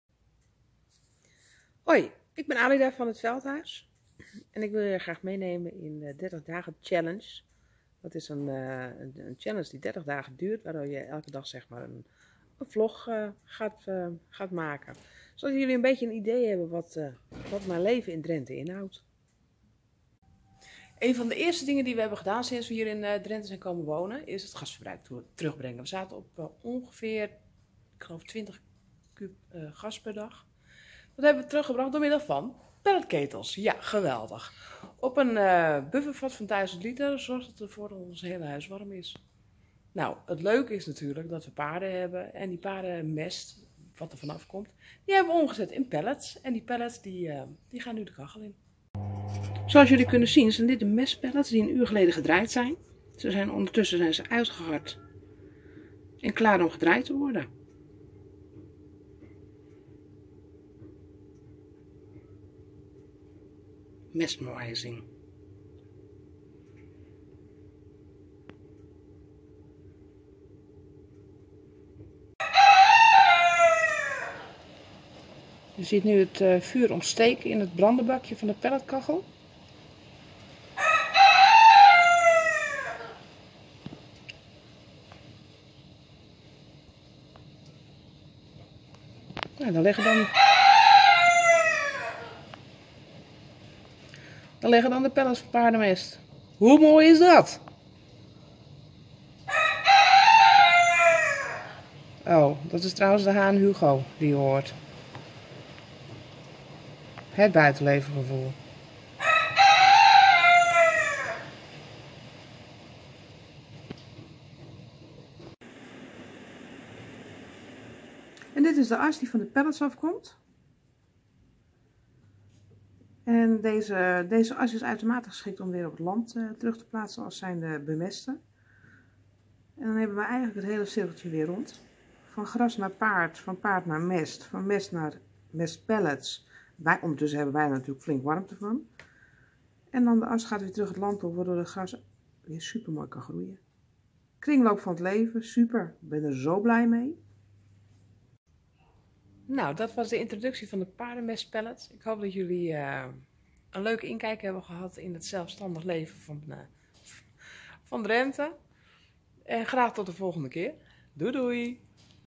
Drenthe ... vlog1 Van aardgas naar pellets.
Drie jaar geleden zijn we met het gezin en de dieren van Zaanstad naar Klazienaveen verhuisd. Deze vlog gaat over het verminderen van aardgas dmv pellets van paardenmest. hetveldhuys biobased pellets recycling paarden warmte 🔥🔥